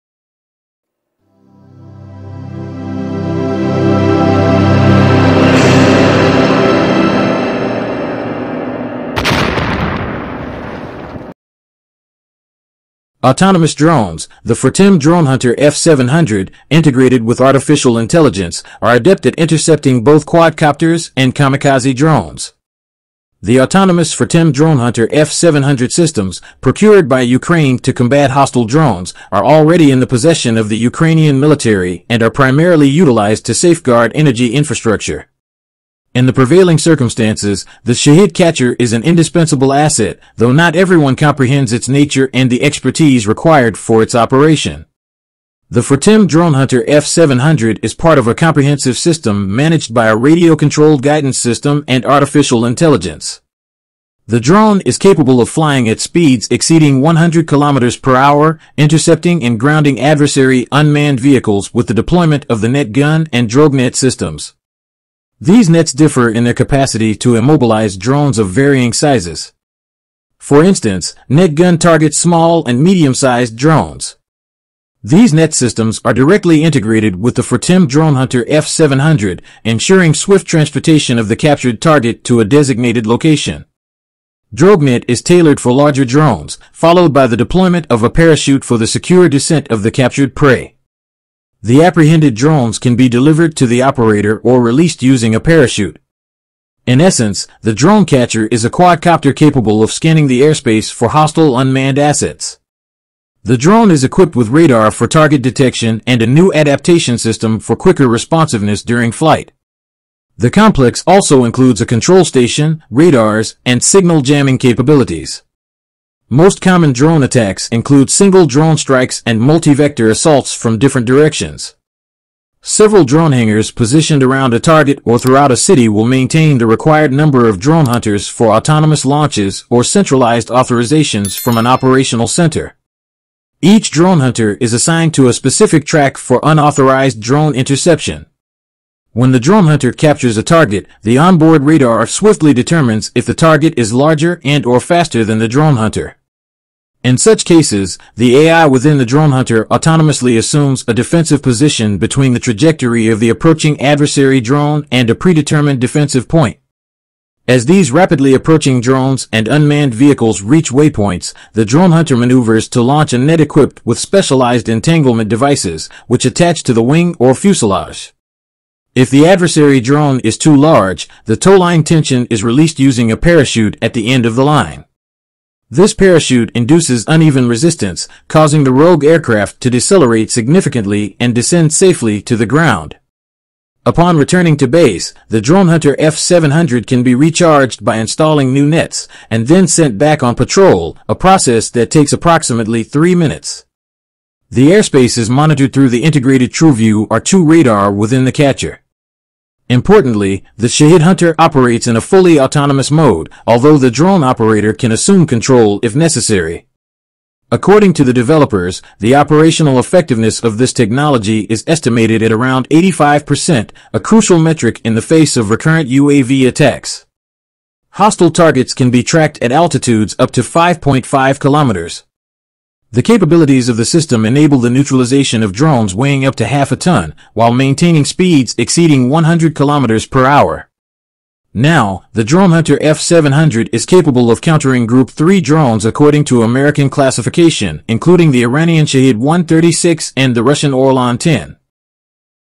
DroneHunter F700
DroneHunter F700 is a state-of-the-art defense technique that protects US strategic facilities.